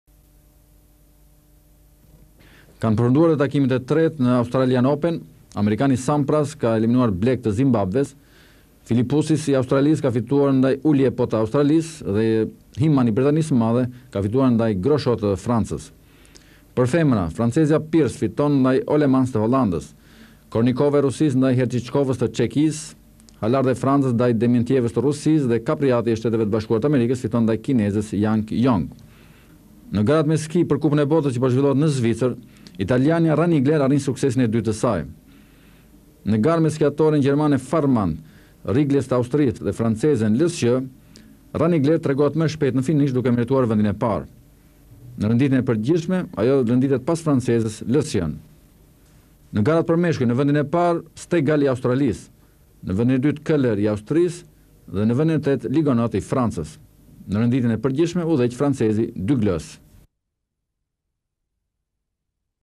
Sport bota tenis, gara me ski - (21 Janar 2000)
Kanë përfunduar takimet e treta në Australian Open në tenis.  Amerikani i Sampras ka eleminuar Black te Zimbabëve. Philipoussis i Australisë ka fituar me Ille të Australisë dhe Henman i Britanisë ka fituar ndaj Graosjean të Francës.

Në garat me ski për kupën e botës që po zhvillohet në Zvicër, italianja Ranigler arrin suksesin e dytë të saj duke merituar vendin e parë. Në renditjen e përgjithshme ajo renditet pas francezes Lusien. Në garat ski për meshkuj në vend të parë del Australia, në vendin e dytë Caler i Austrisë dhe në vendin e tretë Franca./tvklan.al